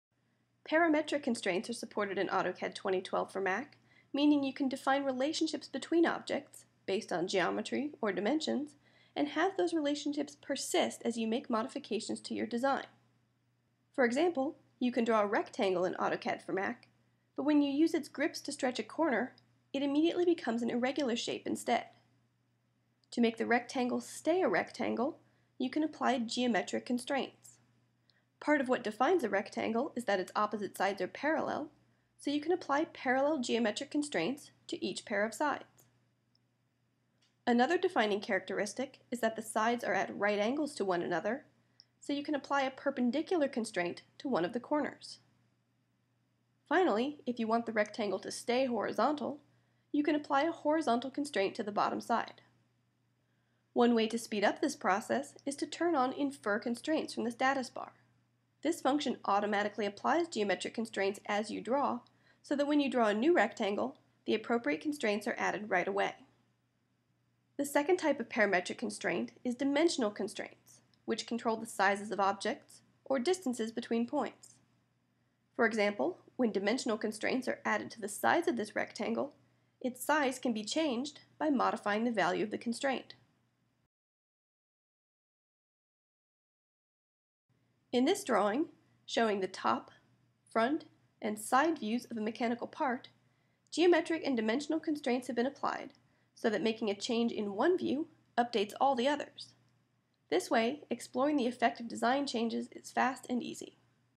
AutoCAD 2012 for Mac - Parametric Constraints
Learn how to define relationships between objects with parametric constraints, one of the new features in the AutoCAD 2012 for Mac.